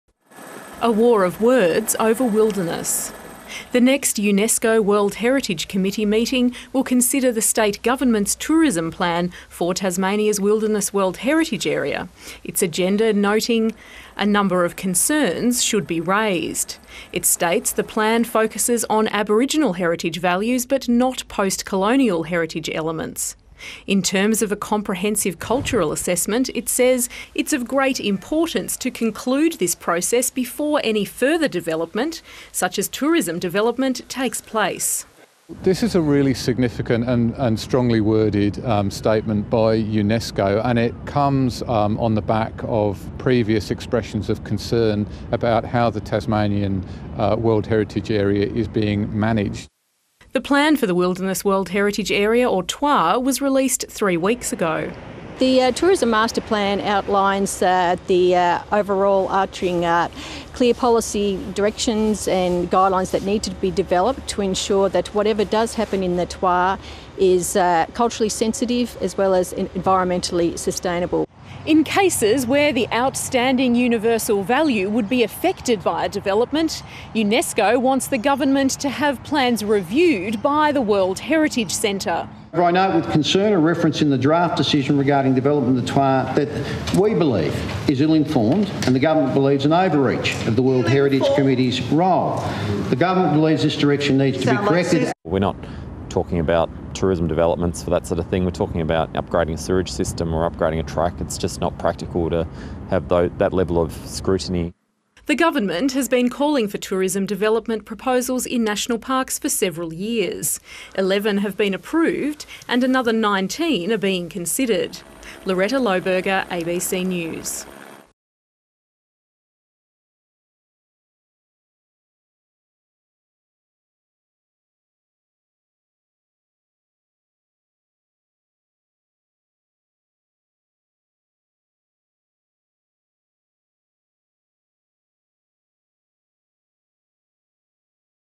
UNESCO calls on Tasmania to pause wilderness development | ABC News
The international body which governs World Heritage areas calls on the Tasmanian Government to halt tourism development in the state's wilderness until the impacts are fully understood.